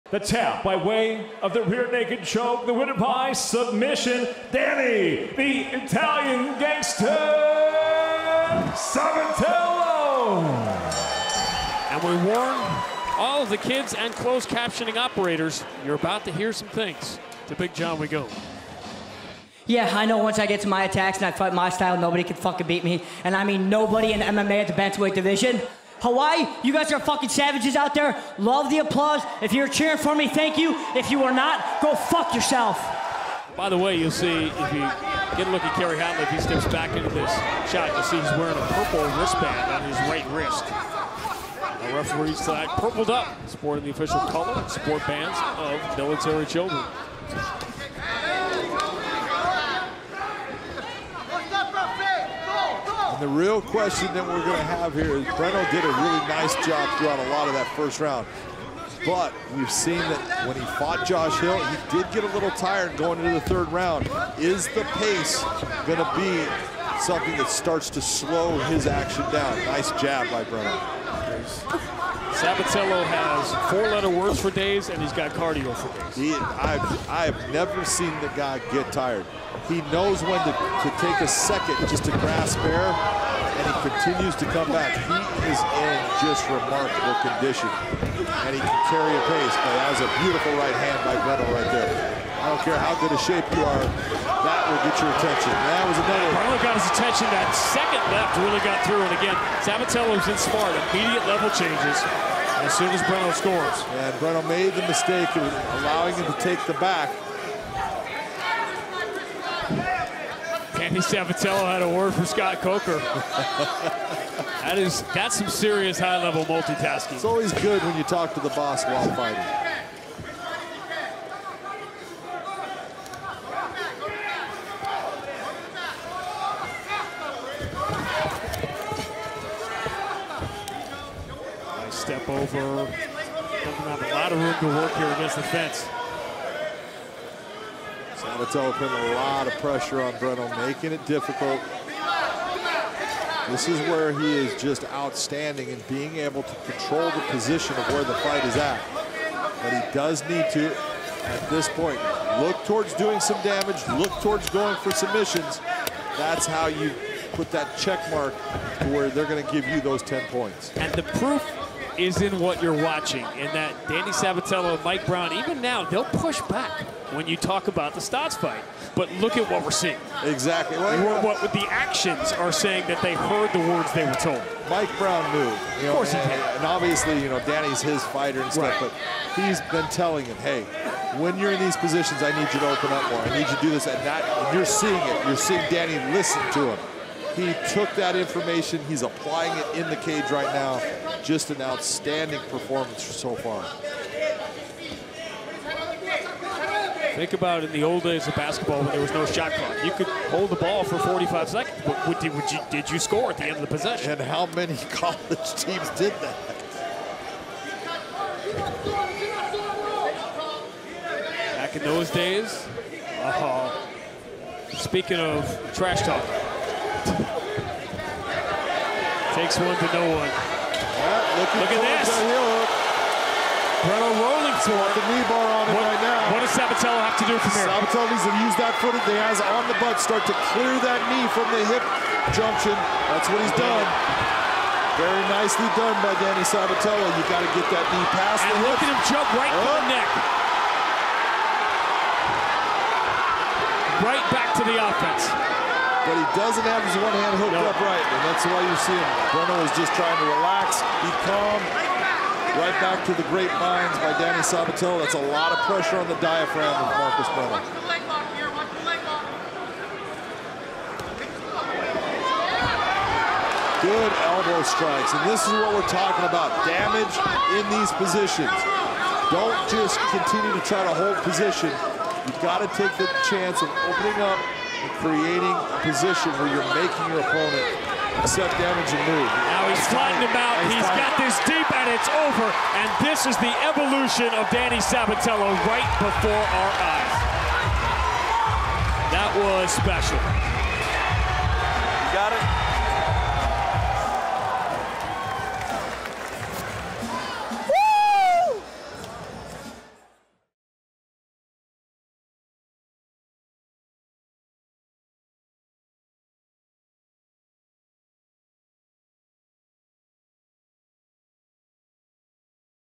💥 Danny Sabatello RETURNS to Bellator 301! | Bellator MMA
The rematch we all have been waiting since 2022. The Italian Gangster is back at Bellator 301 to fight against Raugeon Stots. Here is Sabatello's win against Marcos Breno at Bellator 294.